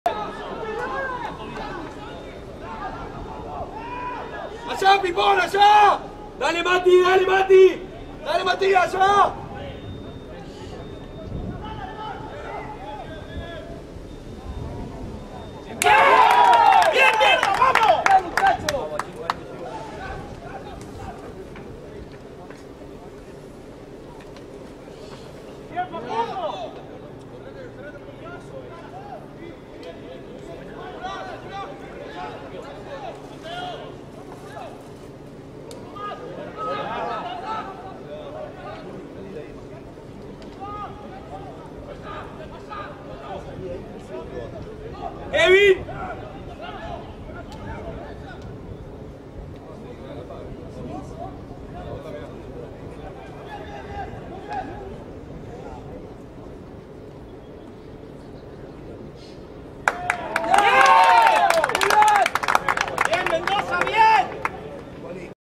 TRY A ROSARIO
Mendoza le gano a Rosari por 22 a 10 en la primera fecha del Argentino M18 2019.